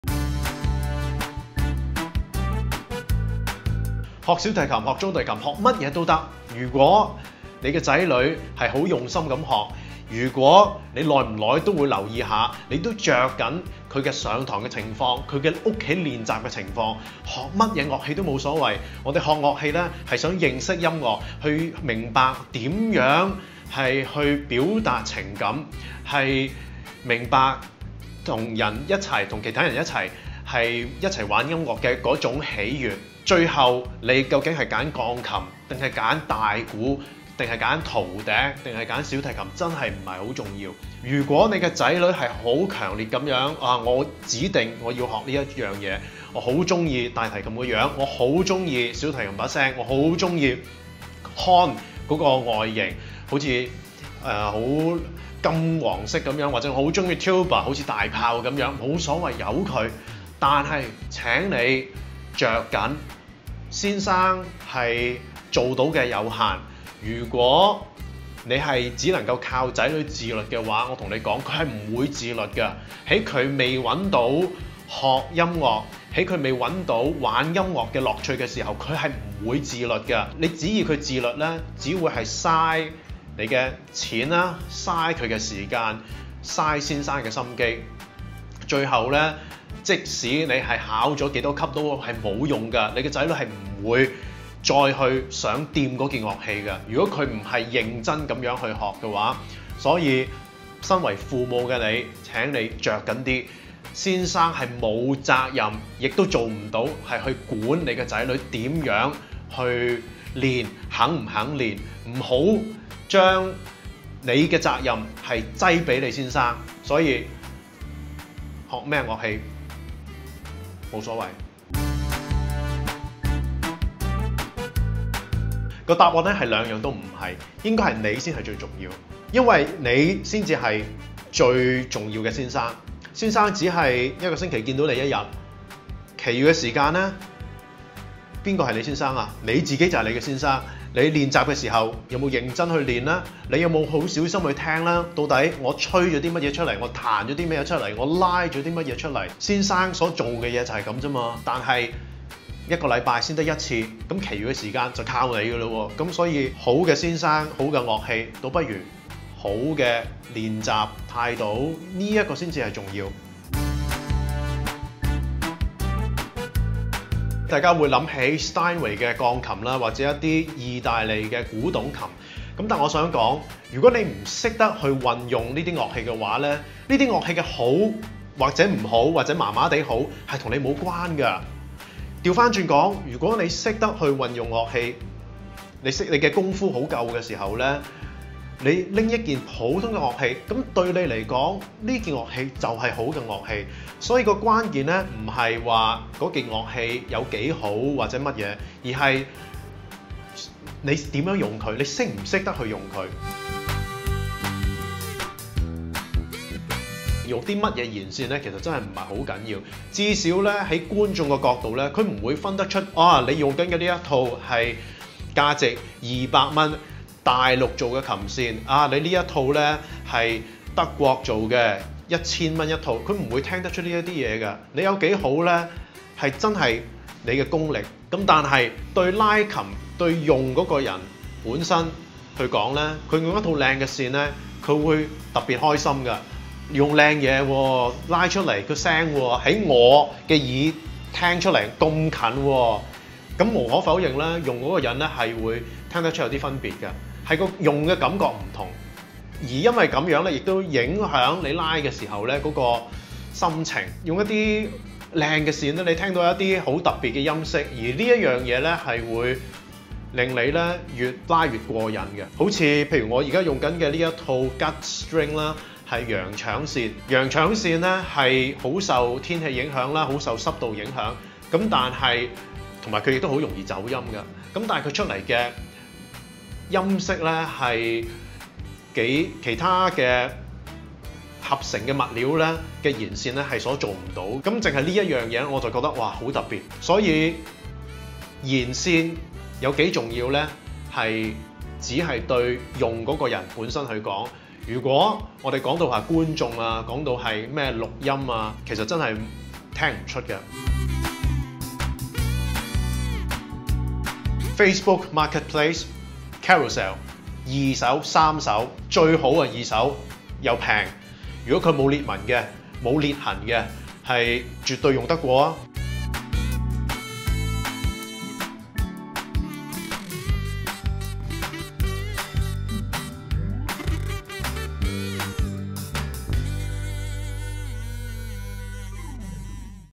【中文字幕】為什麼你不需要一把好琴｜Q&A 3
0:00 該讓子女學什麼樂器？
2:01 樂器重要？還是先生重要？
2:43 我需要一把好琴嗎？
3:23 該用高級的弦線嗎？
5:00 該在哪裏買小提琴？

葉紹羲畢業於澳洲國立大學，後赴蘇格蘭深造。曾效力澳洲坎培拉交響樂團、蘇格蘭歌劇院樂團、皇家蘇格蘭國家樂團。自 2005 年為香港小交響樂團全職樂手，於多間學校任教小提琴和中提琴。